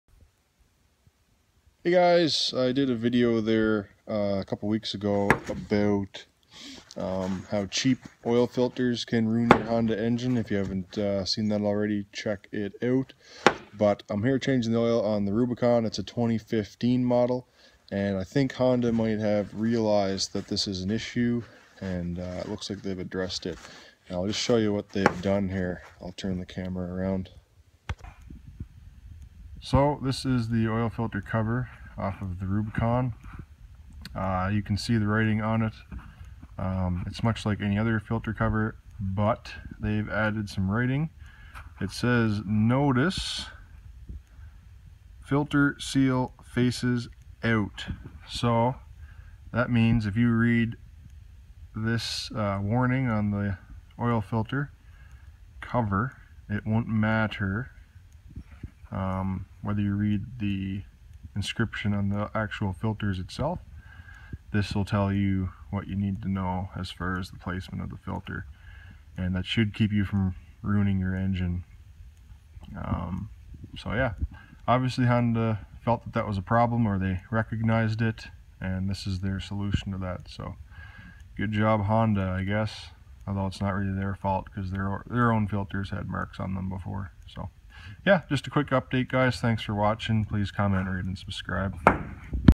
HONDA'S SOLUTION TO THE OIL FILTER PROBLEM REVEALED
This is an update to the cheap oil filter video showing you Honda's solution to help reduce the engine failures associated with cheap Chinese oil filters.